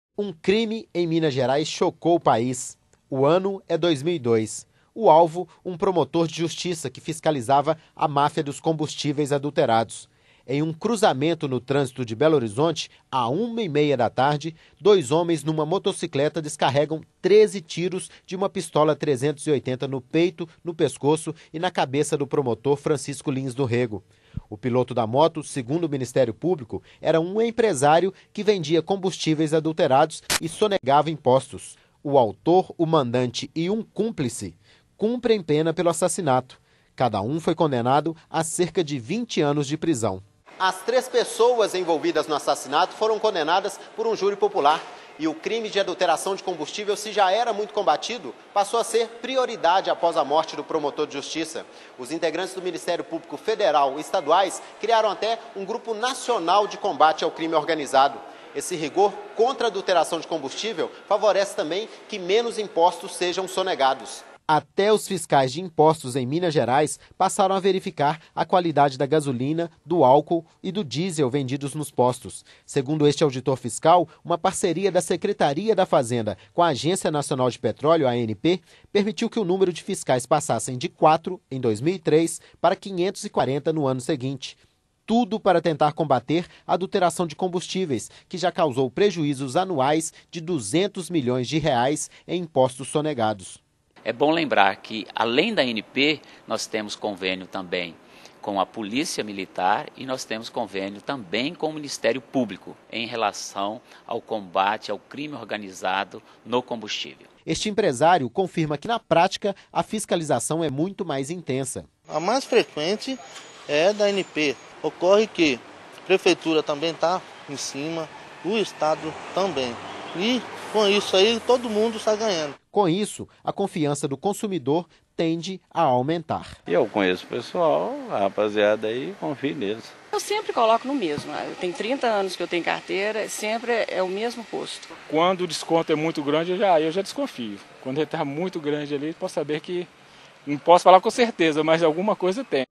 Fiscalização dos Combustíveis
O assassinato de  um promotor de Justiça de Minas Gerais que estava montando um cerco à máfia que vendia combustível adulterado no Estado e o aumento do rigor no combate ao combustível adulterado.